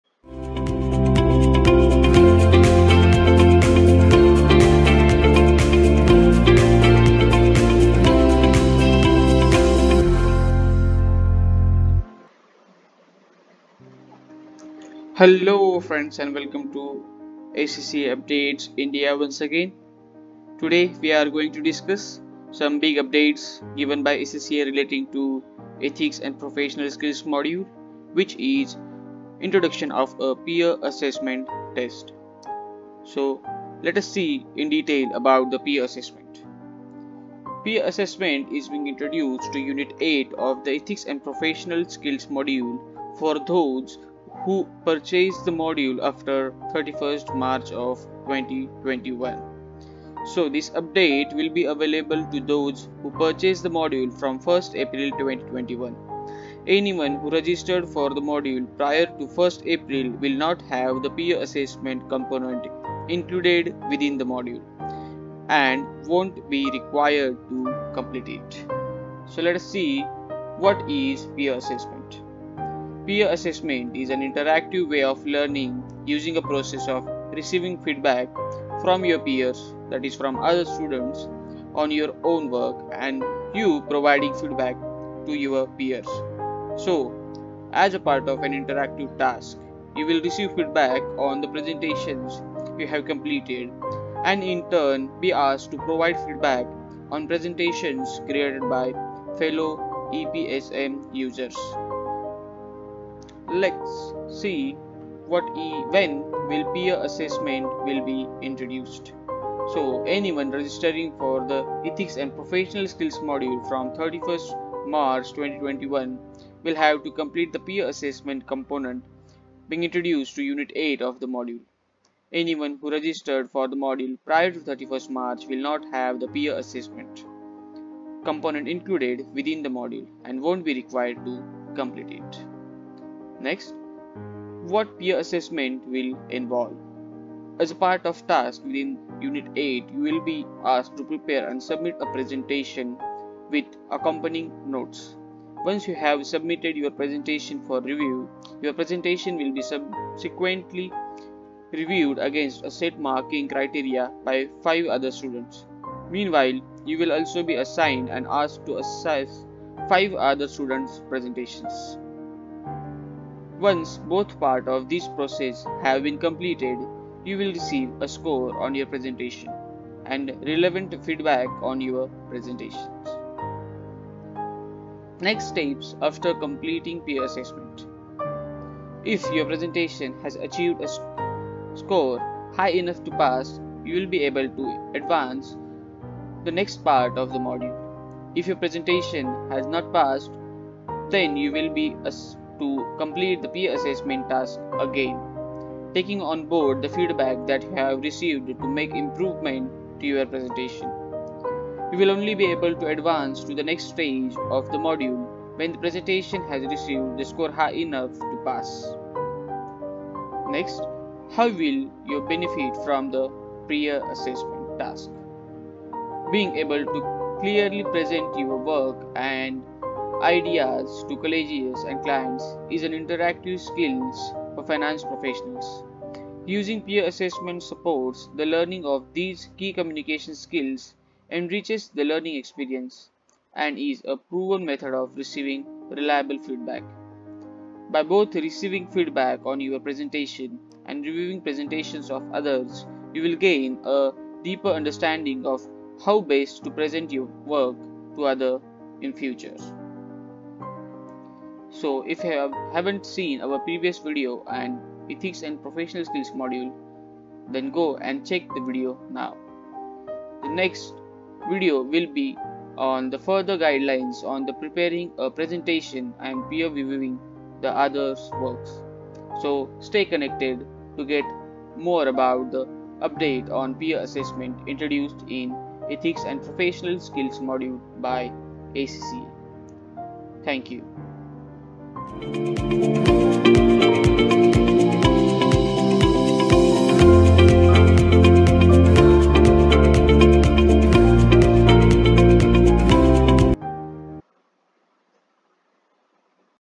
Peer Assessment in EPSM | Big Update in EPSM from April 2021
BIG UPDATE IN ETHICS AND PROFESSIONALS SKILLS MODULE.

Peer assessment is being introduced to Unit 8 of the Ethics and Professional Skills module for those who register for the the module from 1 April 2021. Find out more about what this means and how peer assessment will support your learning.